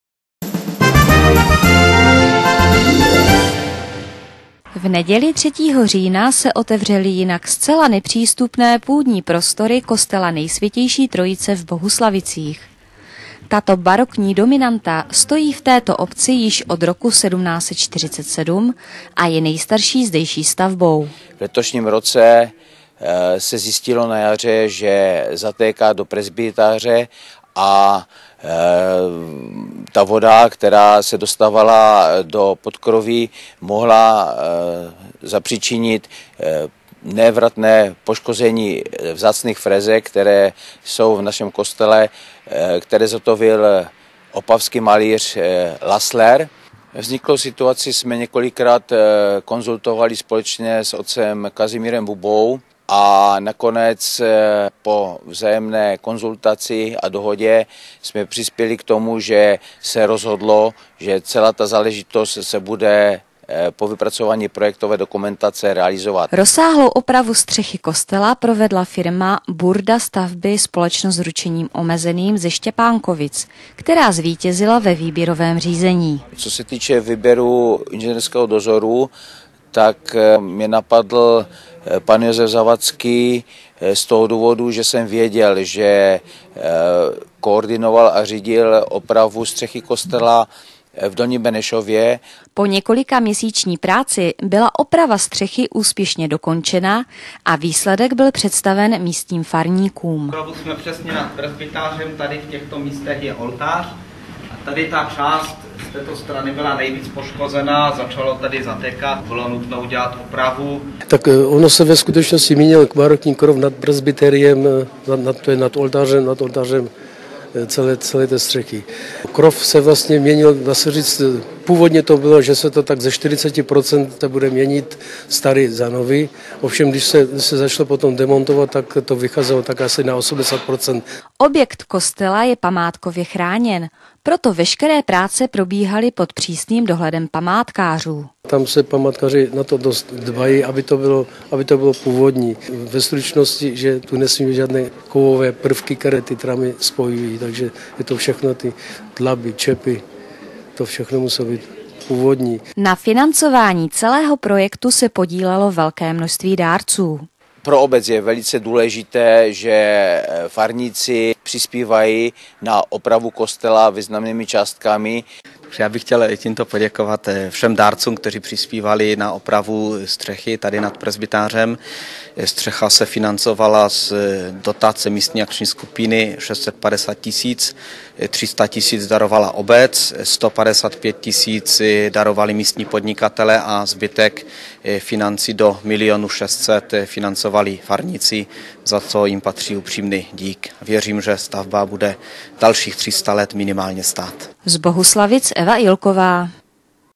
Den otevřených dveří v kostele v Bohuslavicích
V neděli 3. října se otevřely jinak nepřístupné půdní prostory kostela Nejsvětější Trojice v Bohuslavicích.